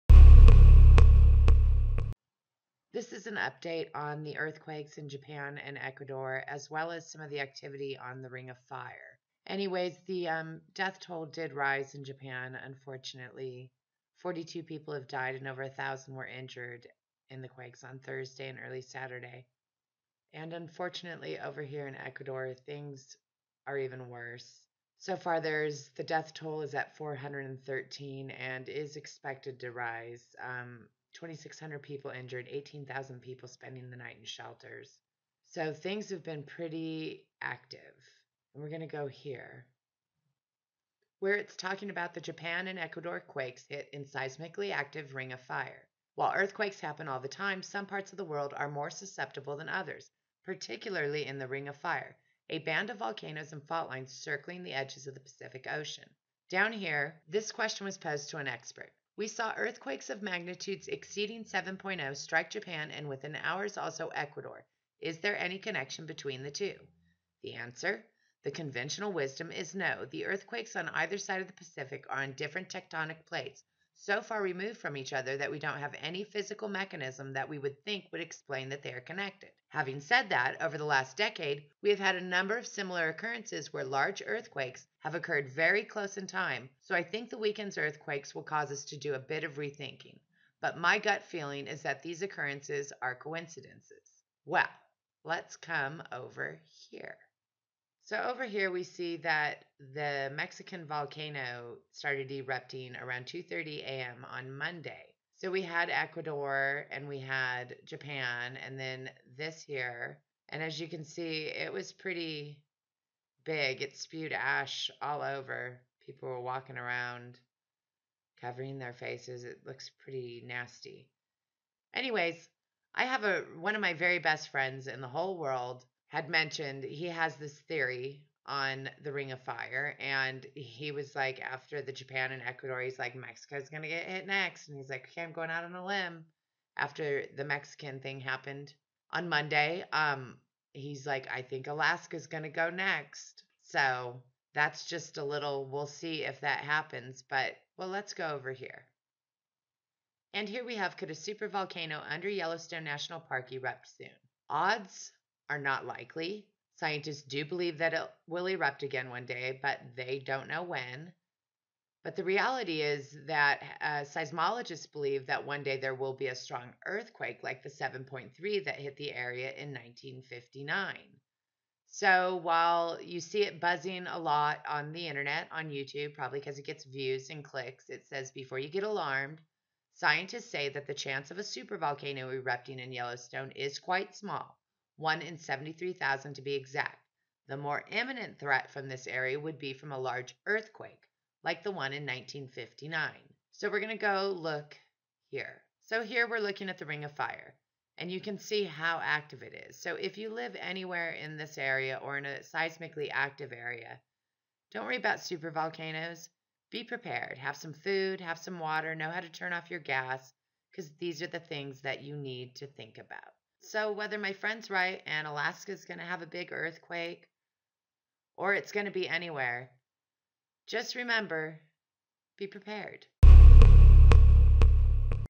Active Ring Of Fire! Be Prepared! Ecuador Death Toll Rises
The Pacific Ring Of Fire is extremely active and causing some  scientists to rethink the links between the different events. Yellowstone continues to trend for the super-volcano however it's a large earthquake that is the more imminent threat.